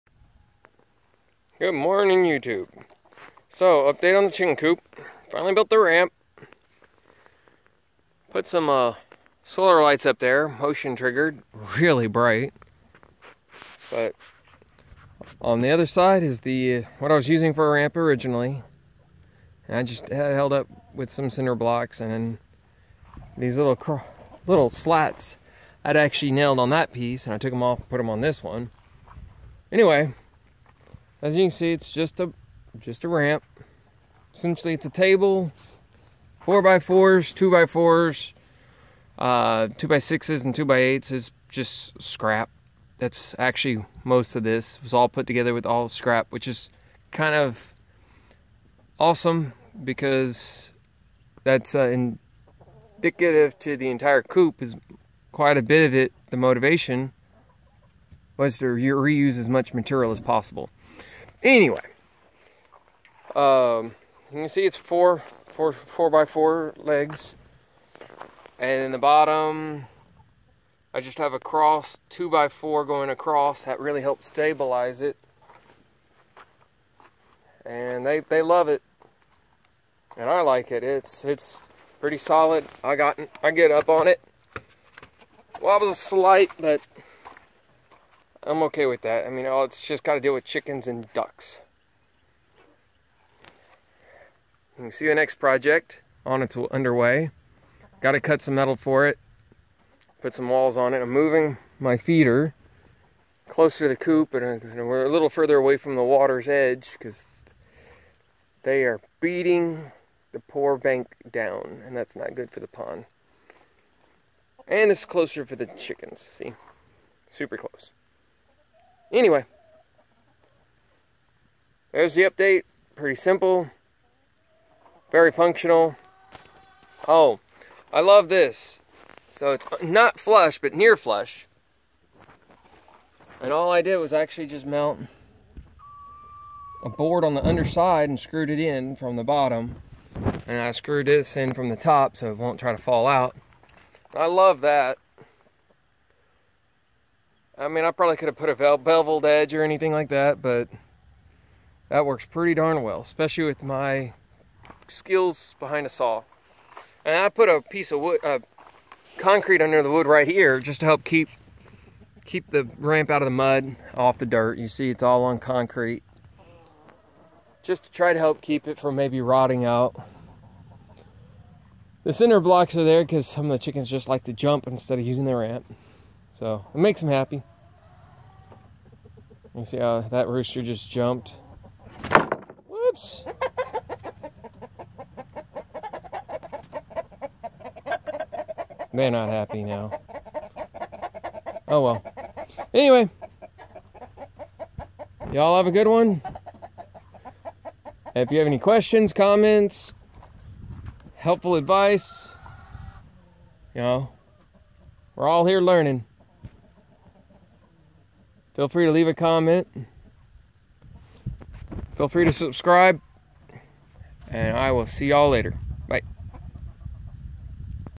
Chicken Coop - Day 5 - Ramp
I finally got around to making an update video of the chicken coop ramp. The weather has been awful and I've been using my outside time on other projects. Hint, I am building a better chicken/duck feeder cover and moving it closer to the chicken coop. The original ramp was just a board held up by concrete blocks, it was meant to be temporary till I could build something more solid and more permanent. It's basically a short table with a board for a ramp.